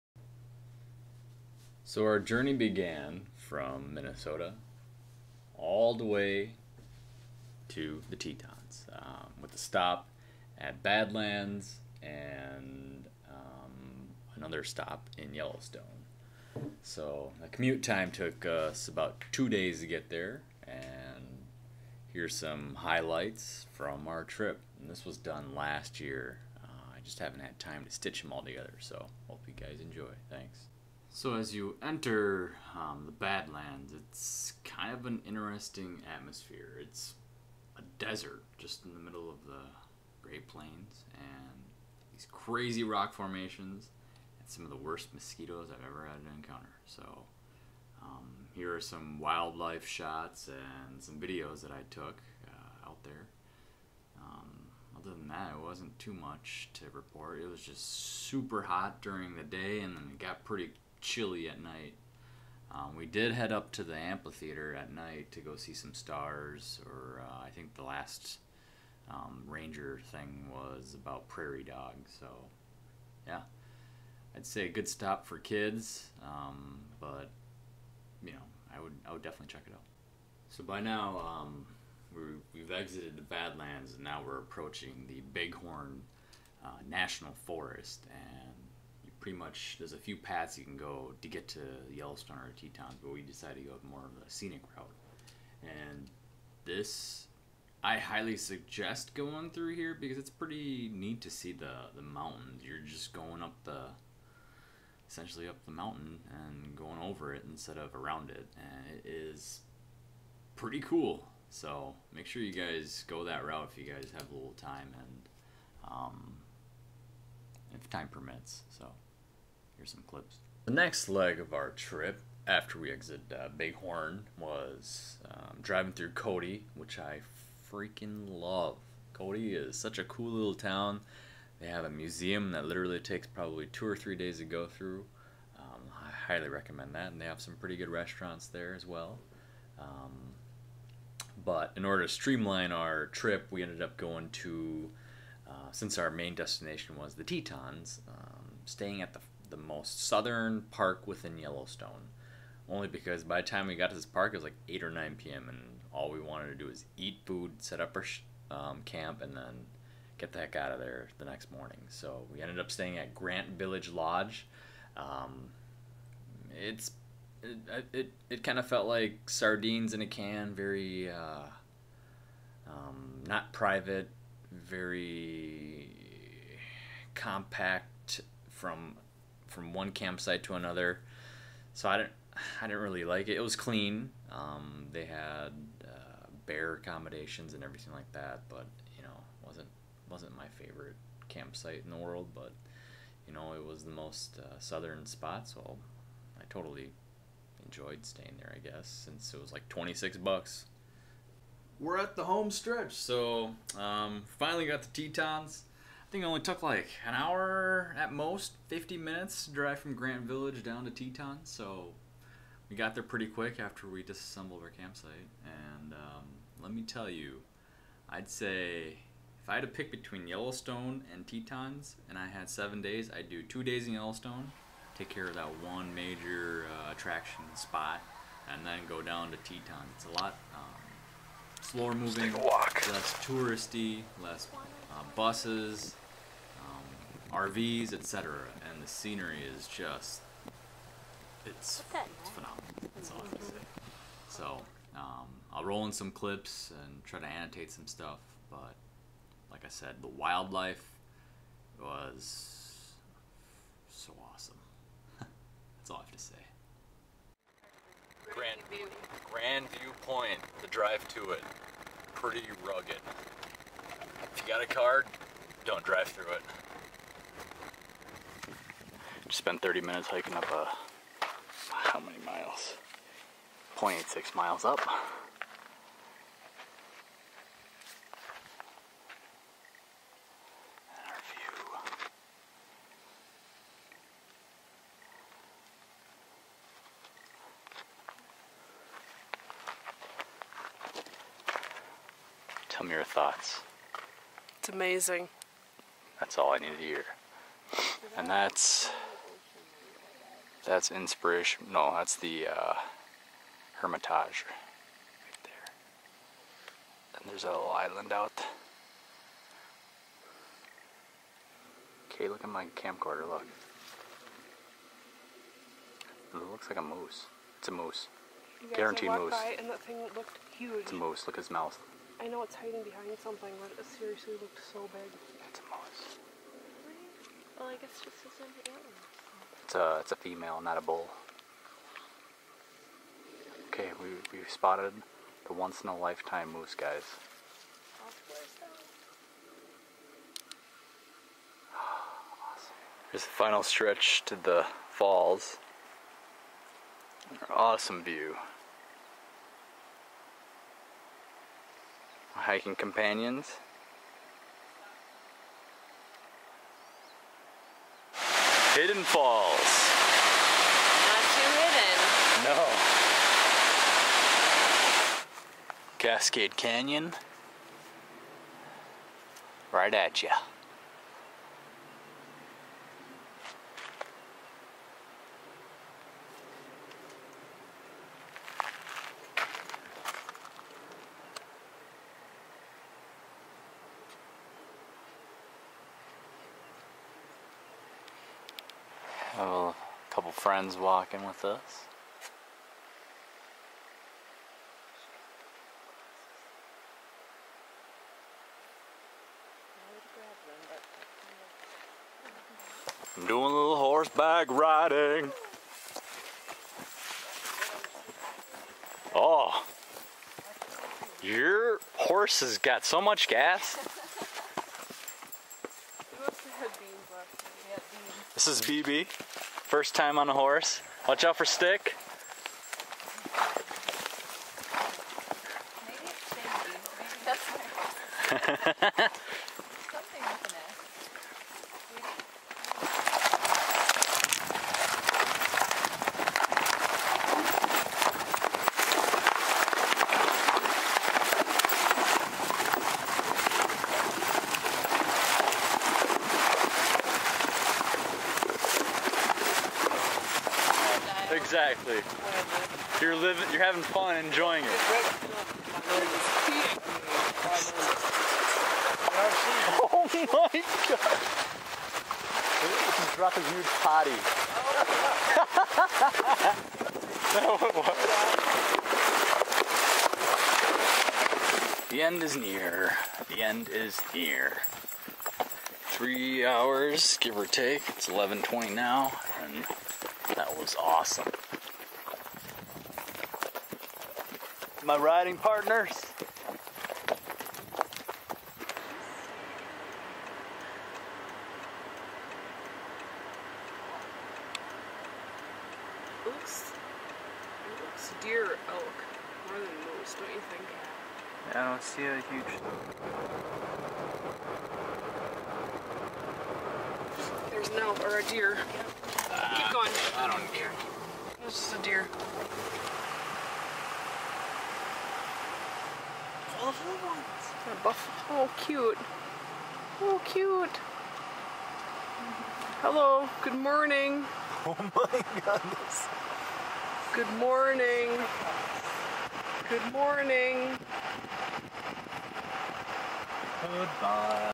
Road Trip MN to Badlands to Bighorn to Grand Tetons by onza04
Road trip MN to the Tetons~!

//Your support equals more videos. Please click like if you thought the video was informative.